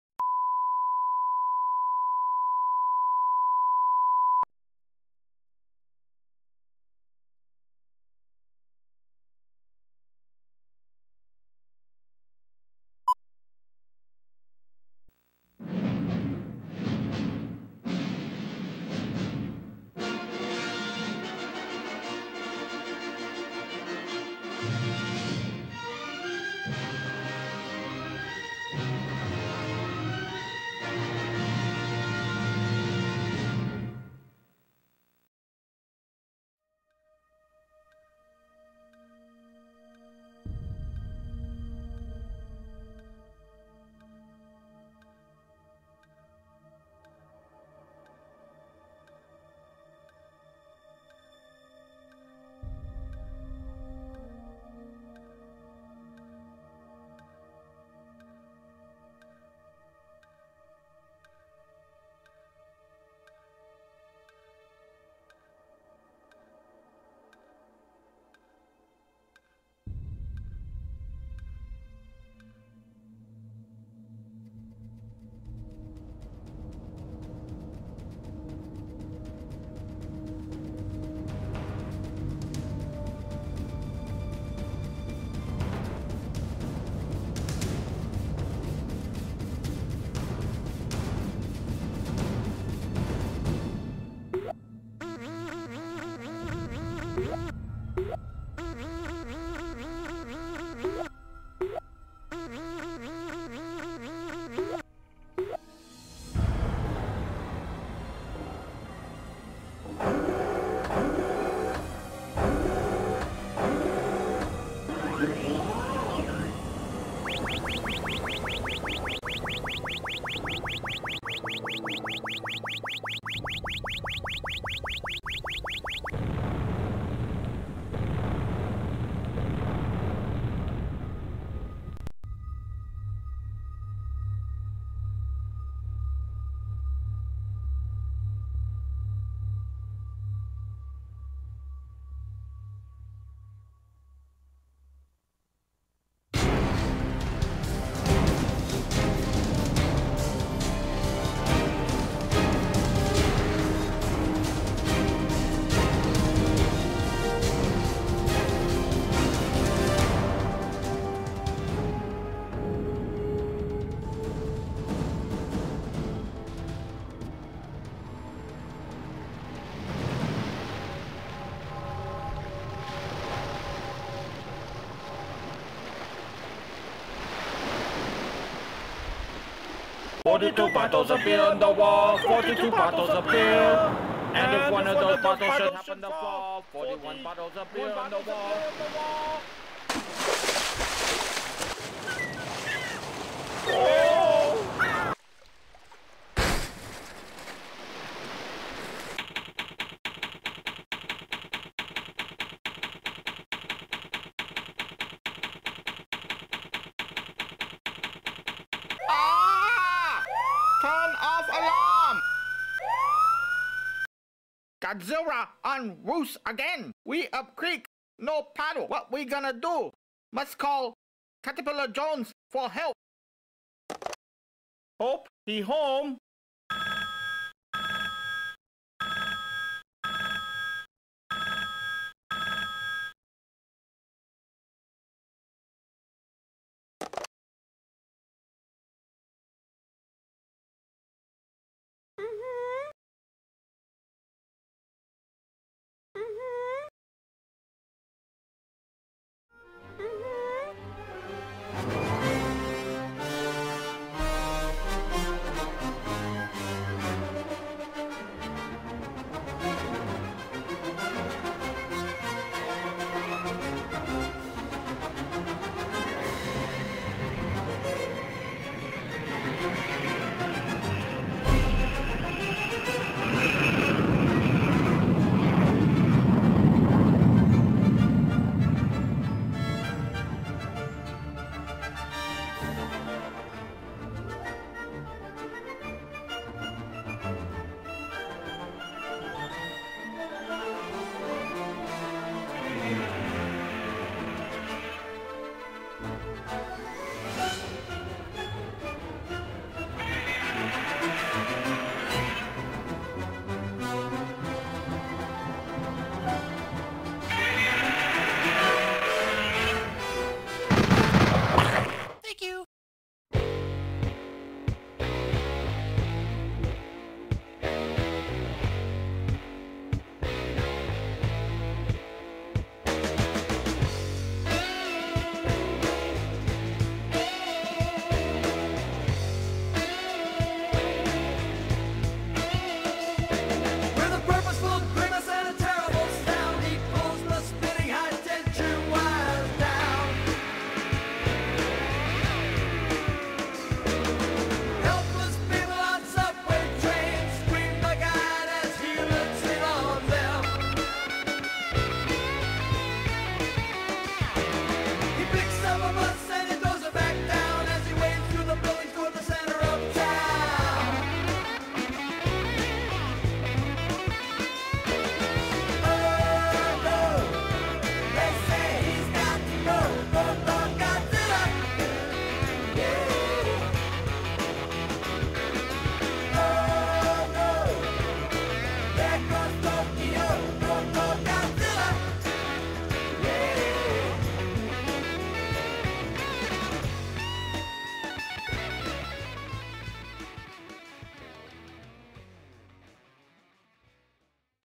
Godzilla vs Caterpillar Jones
Godzilla on the loose again and it will Take Caterpillar Jones to defeat him.